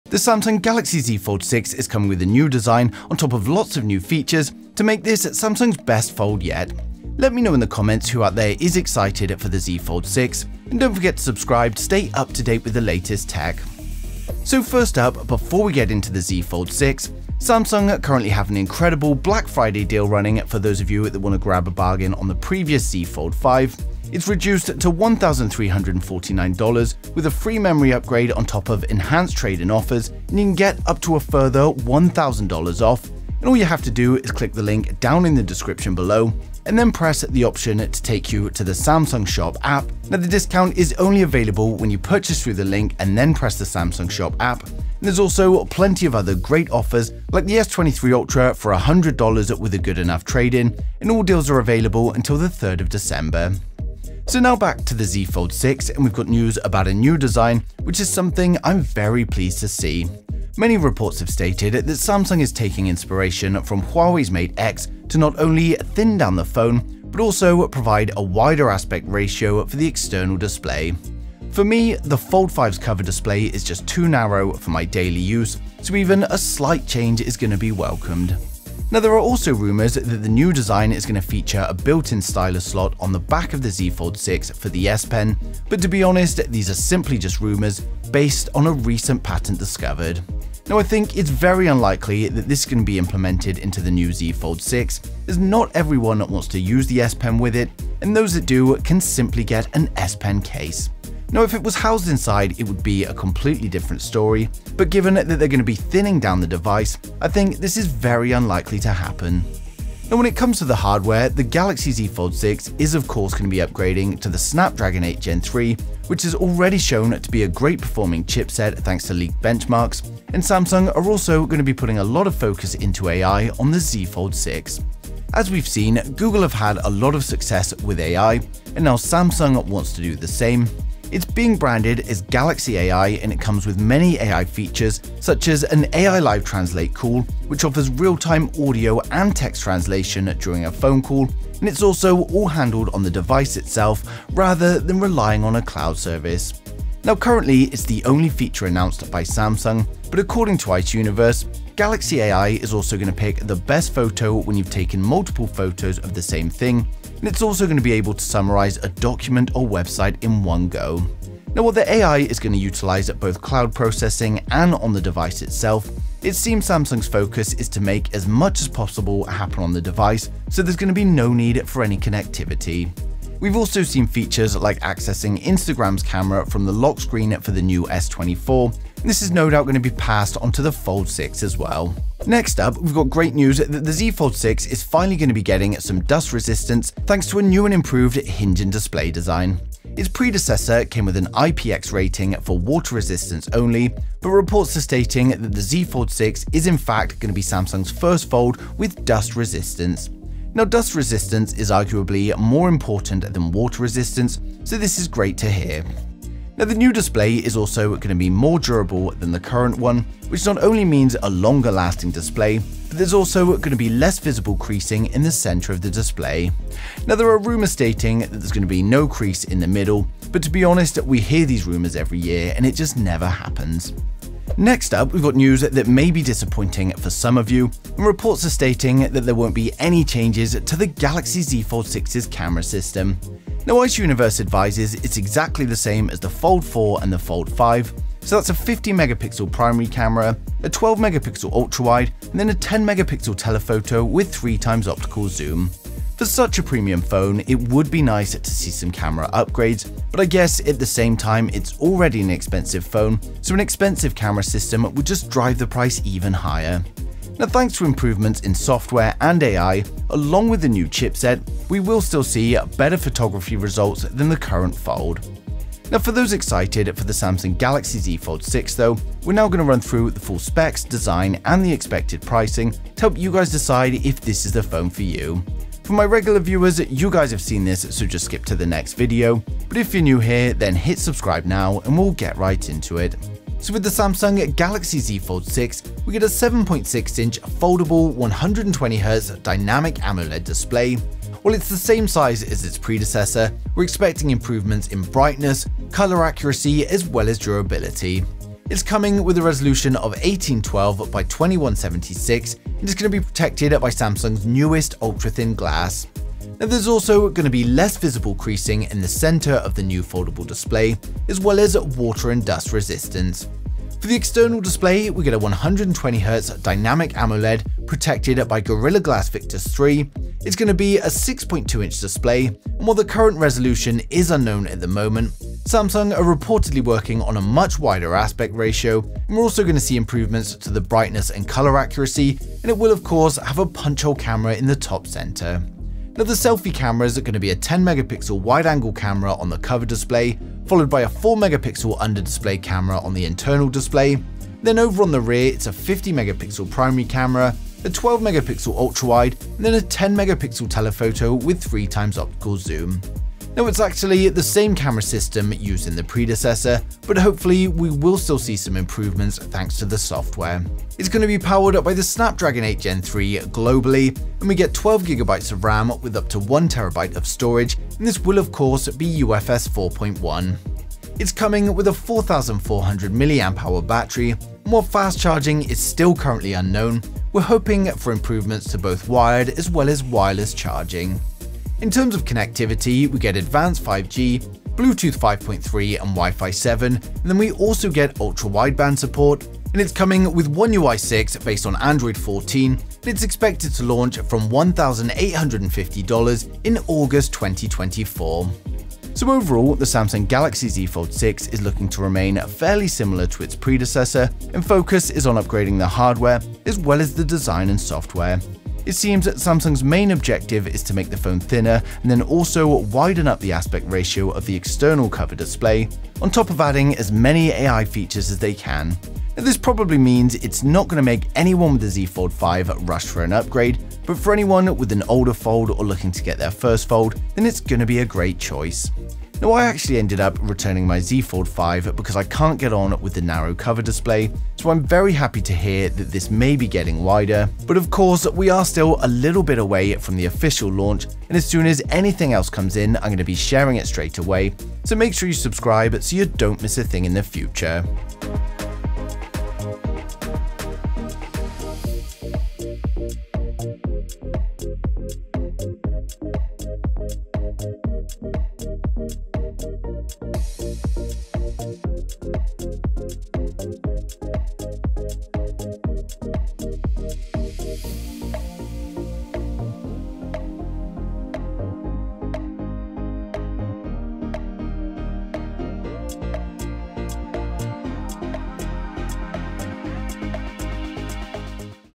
Samsung Galaxy Z Fold 6 - Epic New Design!
The Samsung Galaxy Z Fold 6 is finally coming with the design we've all been waiting for!

The Samsung Galaxy Z Fold 6 is the new flagship phone from Samsung bringing many upgrades over the predessesor. With upgrades to the hardware, software, design & features, the Samsung Galaxy Z Fold 6 is sure to be a success. In todays video we cover the specs, design, launch date & price of the Samsung Galaxy Z Fold 6. We have the trailer video and hands on unboxing review of the Z Fold 6.

Concept Video Courtesy Of:
TT Technology